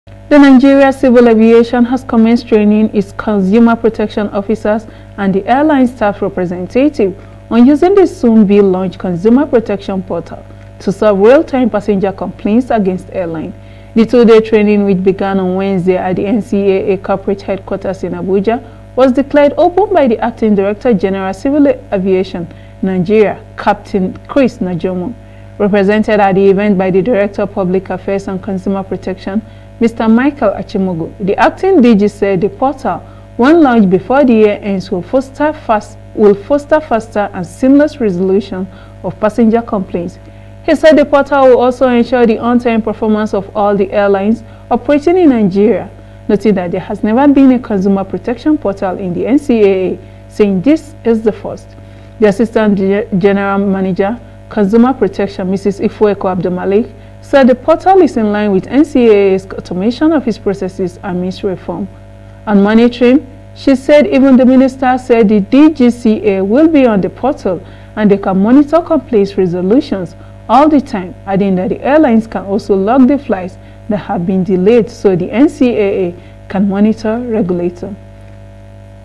NCAA’s Consumer Protection Portal Will Hasten Complaints Resolution – Capt, Najomo
The Nigeria Civil Aviation (NCAA) has commenced training its consumer protection officers (CPOs) and the airlines' staff representative on using the soon-be-launched consumer protection portal to solve real-time passenger complaints against airlines.

The two-day training which began on Wednesday at the NCAA corporate headquarters in Abuja was declared open by the Acting Director General, Civil Aviation, Nigeria captain Chris Najomo.

Represented at the event by the Director Public Affairs and Consumer Protection, Mr. Michael Achimugu, the Ag.DG said the portal when launched before the year ends will foster faster and seamless resolution of passenger complaints.

He said The portal will also ensure the on-time performance of all the airlines operating in Nigeria noting that There has never been a consumer protection portal in the NCAA saying This is the first.

The Assistant General Manager, Consumer Protection, Mrs. Ifueko Abdulmalik said the portal is in line with NCAA’s automation of its processes amid reforms.

On monitoring, she said even the Minister and the DGCA will be on the portal and they can monitor complaints resolutions all the time adding that the airlines can also log the flights that have been delayed so the NCAA can monitor regulatory compliance